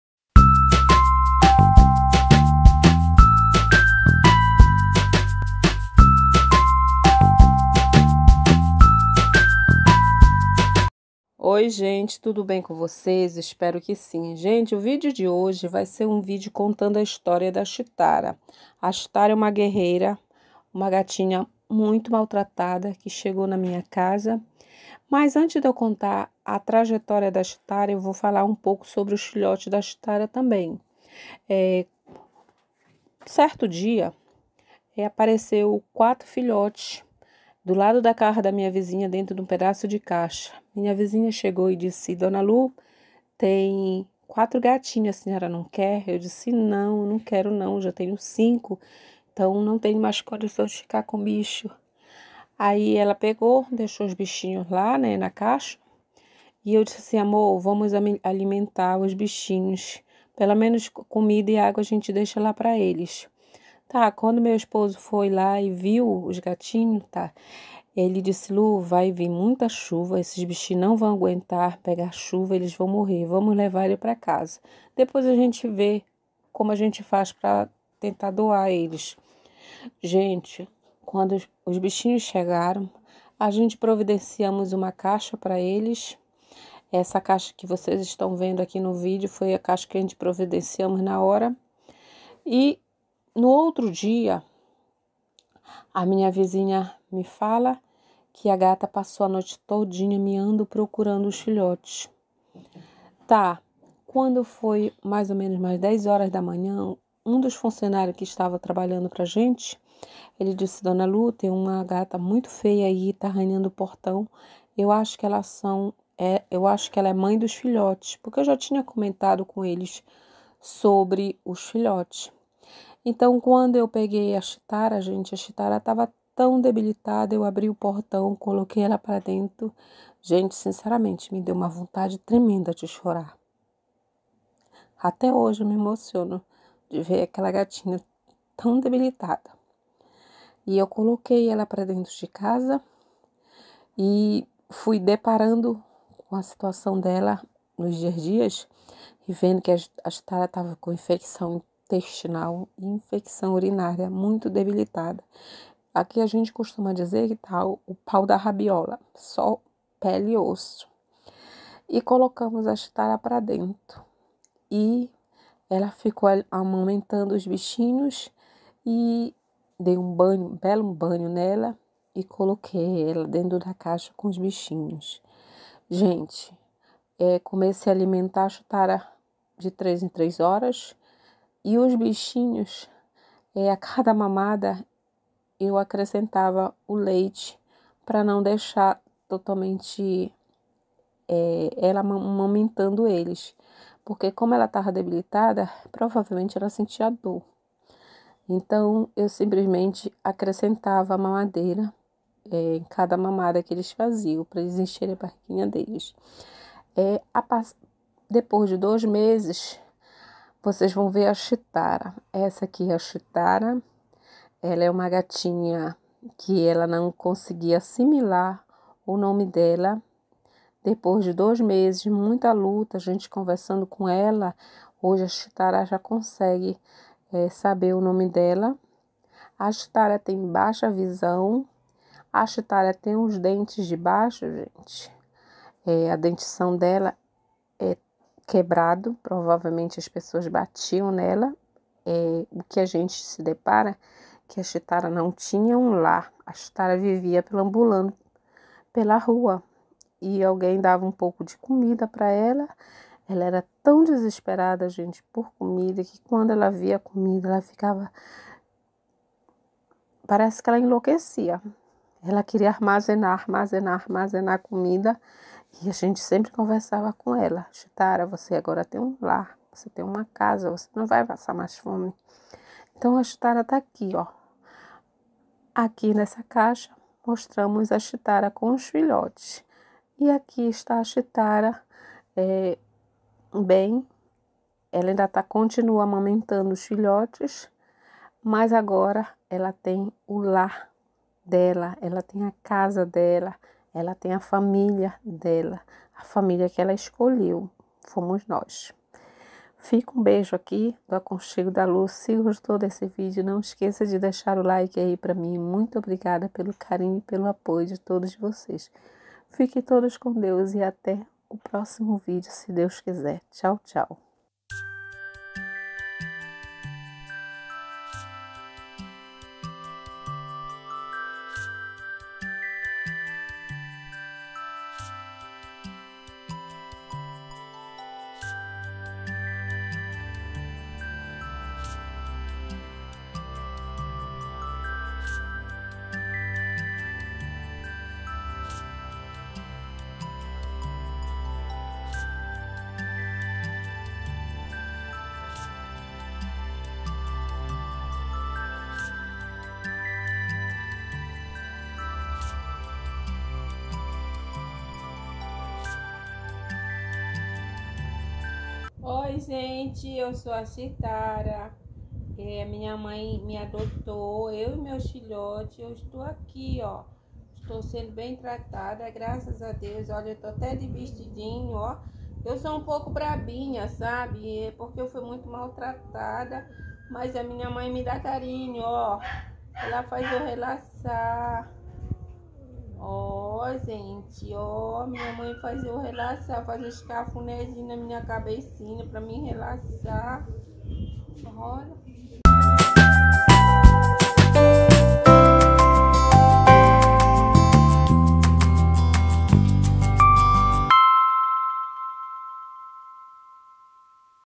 cheetara e seus filhotes
Oi, gente!
O vídeo de hoje vai ser sobre os gatinhos que nos resgatamos, a cheetara e seus filhos. Espero que gostem do vídeo.
Rotina, dona de casa, cotidiano, família, dia a dia,